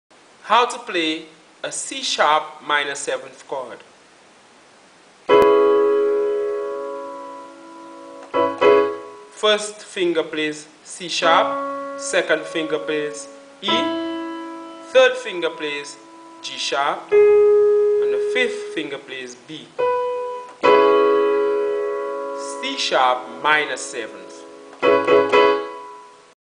How To Play C#m7 Chord (C Sharp Minor Seventh, C#min7) On Piano & Keyboard
Learn how to play the C#m7 chord in this short video. To learn how to play more chords, go How to play the C sharp minor seventh chord on keyboard and piano. C#min7. Notes of C# minor 7th chord are C#, E, G# and B. Fingering is 1 2 3 5.